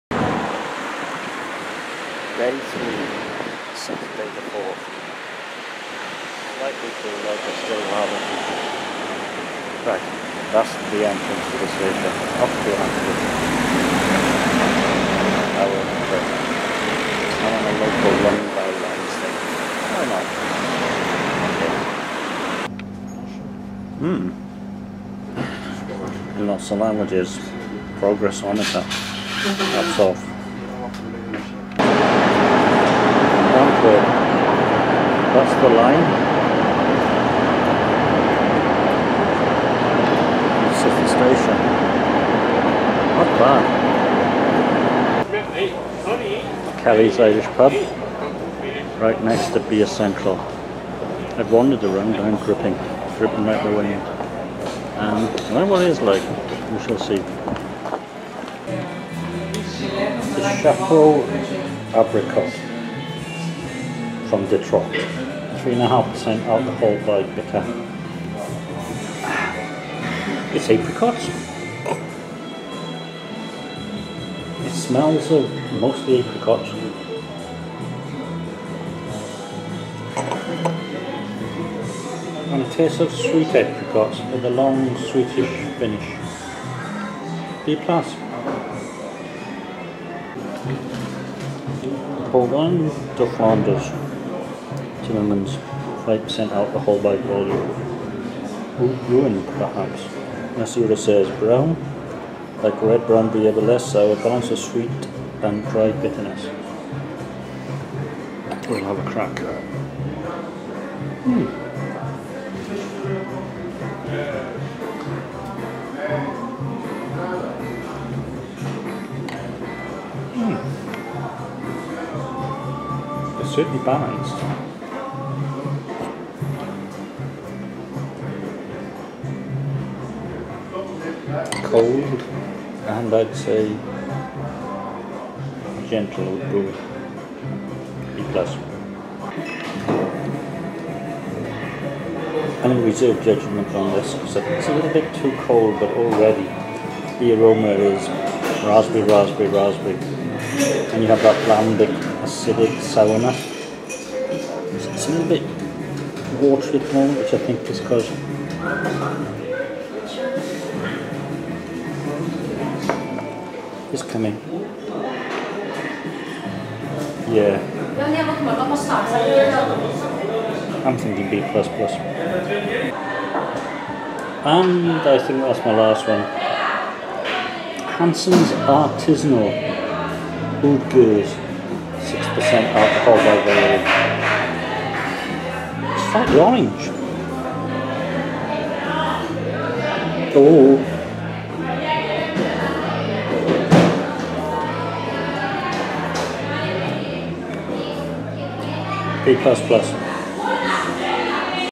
Antwerp in the Heat
Took the train to Antwerp for a wander and a visit to Bier Central. It had mixed reviews, so I thought that I would take a look. Anyway it was boiling on the day that I went there. Good things, aircon, a great beer menu and nice clean toilets. Bad things the Wifi was down, they are not that keen on you ordering from the bar, rather expensive and not all the biers in the menu are in stock. However it is close to the Railway Station so it's worth a visit.